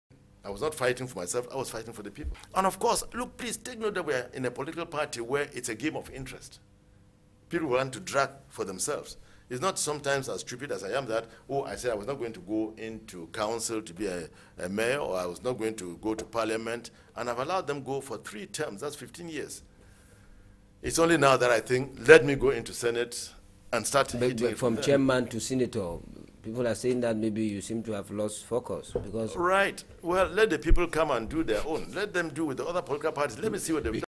FRU NDI game of interest
SDF Chairman Ni John Fru Ndi says he is fighting for the people. At the same time, regrets he has allowed militants go to parliament for three terms. He argues that he has not lost focus after a failed attempt to become senator...Fru Ndi, True to himself...MUST WATCH!